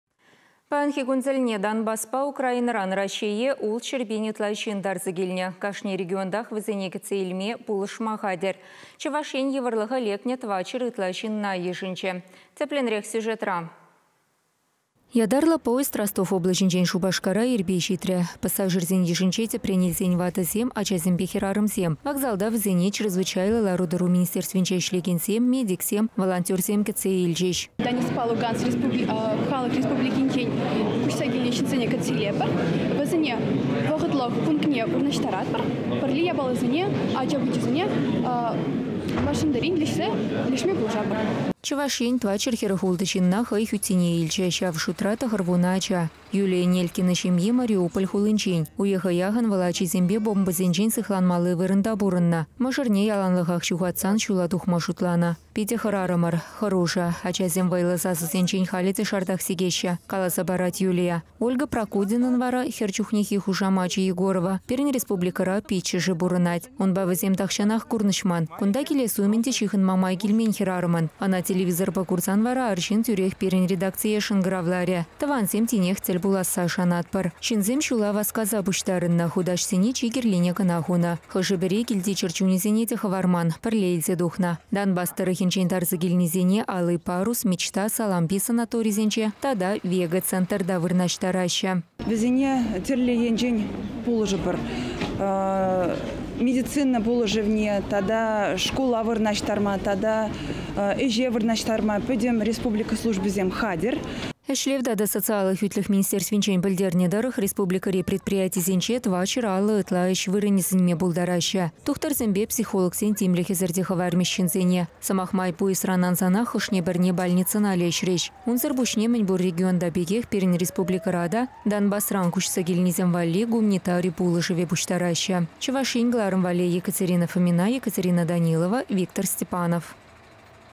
Паянхи кун тӗлне Донбасспа Украинӑран Раҫҫее 600 пин ытла ҫын тарса килнӗ
Кашни регионтах вӗсене кӗтсе илме, пулӑшма хатӗр. Чӑваш Ен йывӑрлӑха лекнӗ 400 ытла ҫынна йышӑнчӗ.